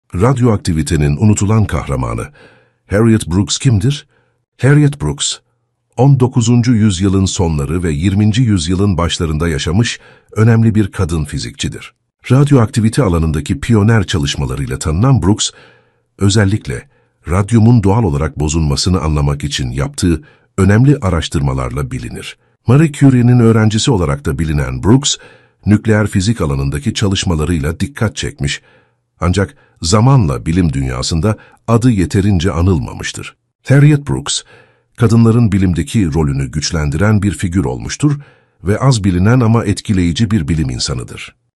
Radyoaktivitenin Unutulan Kahramanı: Harriet Brooks Kimdir?
Harriet Brooks, 19. yüzyılın sonları ve 20. yüzyılın başlarında yaşamış önemli bir kadın fizikçidir. Radyoaktivite alanındaki pioner çalışmalarıyla tanınan Brooks, özellikle radyumun doğal olarak bozunmasını anlamak için yaptığı önemli araştırmalarla bilinir. Marie Curie'nin öğrencisi olarak da bilinen Brooks, nükleer fizik alanındaki çalışmalarıyla dikkat çekmiş, ancak zamanla bilim dünyasında adı yeterince anılmamıştır. Harriet Brooks, kadınların bilimdeki rolünü güçlendiren bir figür olmuştur ve az bilinen ama etkileyici bir bilim insanıdır.